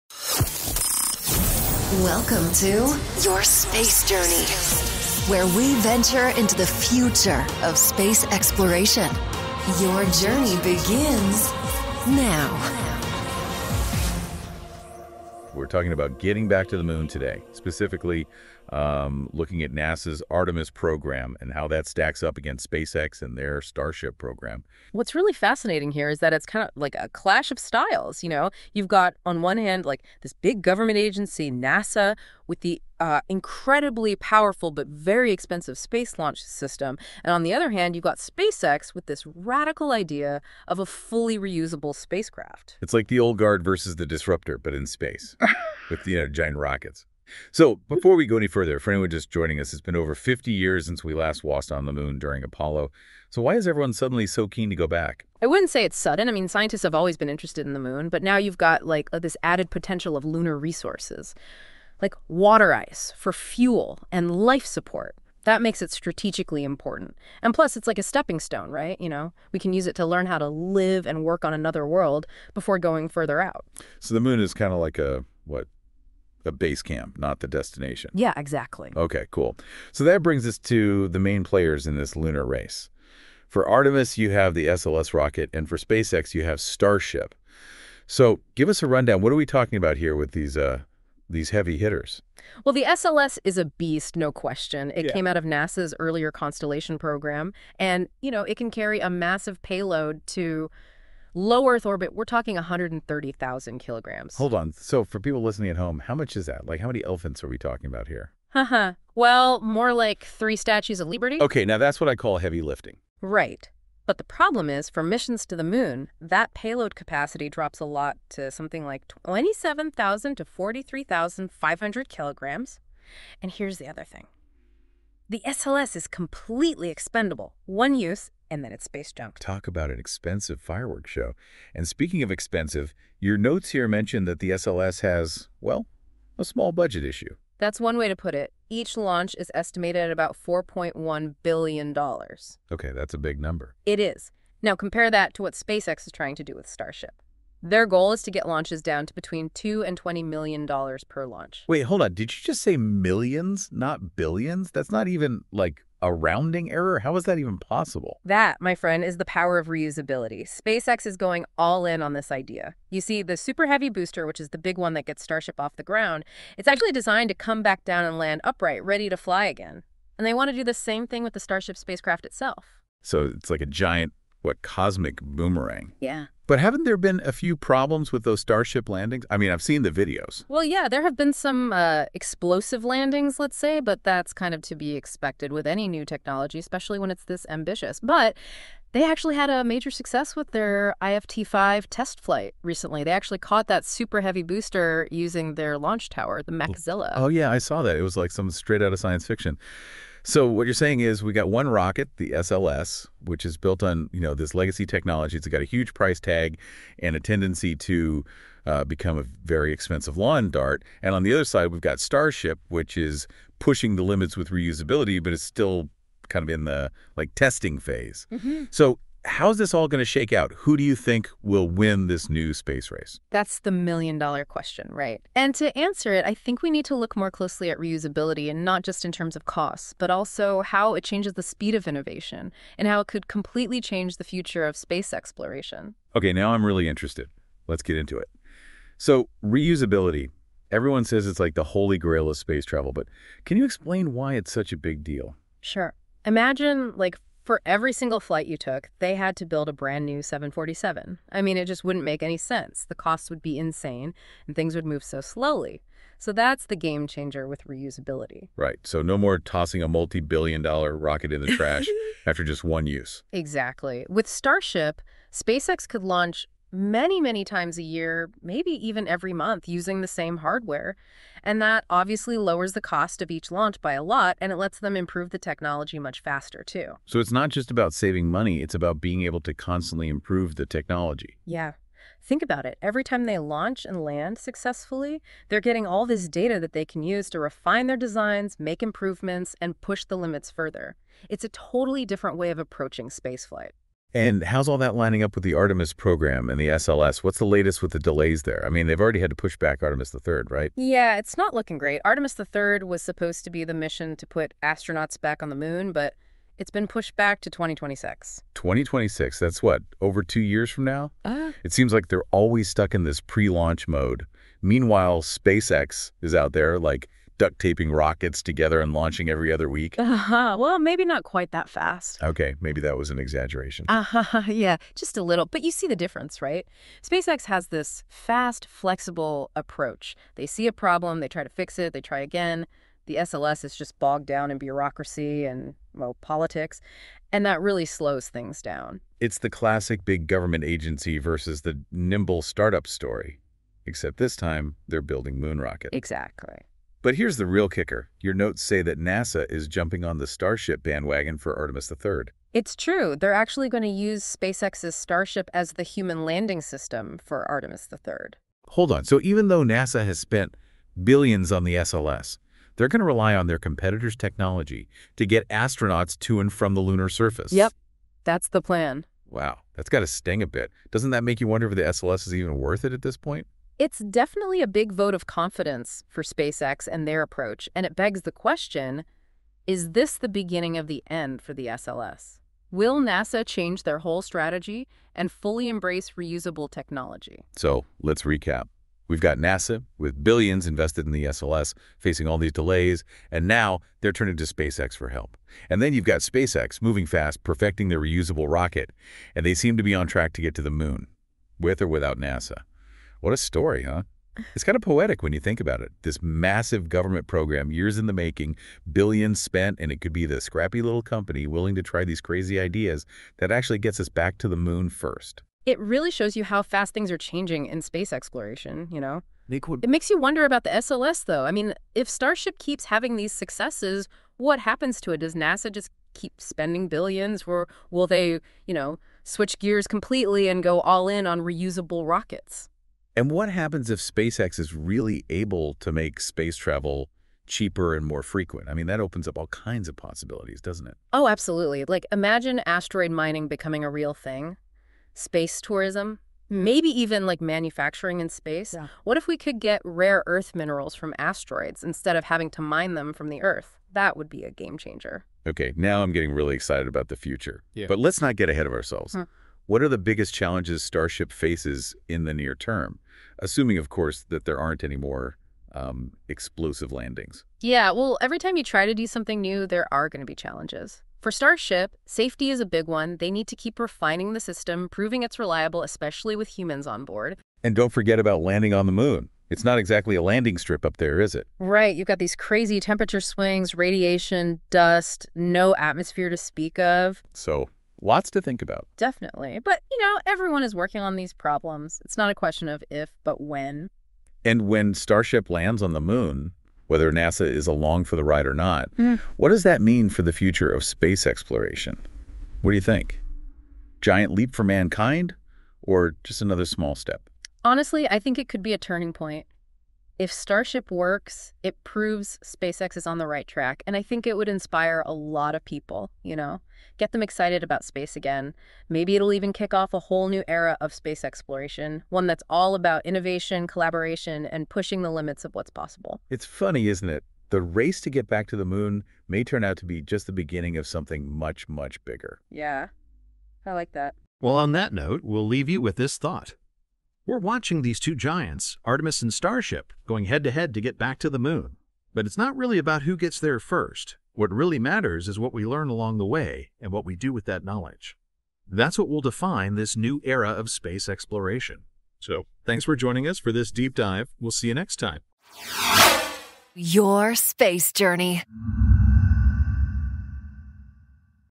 (Podcast) Is NASA's Artemis program still relevant in the age of SpaceX's Starship?
🚀 Is NASA's Artemis program still relevant in the age of SpaceX's Starship? Join us as we explore the future of lunar exploration and the clash between traditional space agencies and innovative private companies.

In this episode, we dive deep into:
• NASA's Artemis program vs SpaceX's Starship: A battle of approaches
• The game-changing potential of reusable rockets
• Cost comparison: SLS's $4.1 billion per launch vs Starship's potential $2-20 million
• How SpaceX's rapid innovation is reshaping the space industry
• The challenges facing both NASA and SpaceX in returning to the Moon
• The future of space exploration: Asteroid mining, space tourism, and beyond

Discover why NASA is now relying on SpaceX for the Artemis III lunar landing and what this means for the future of space travel. Is this the beginning of a new era in space exploration?

Don't miss our exciting discussion on the race back to the Moon and its implications for humanity's future in space!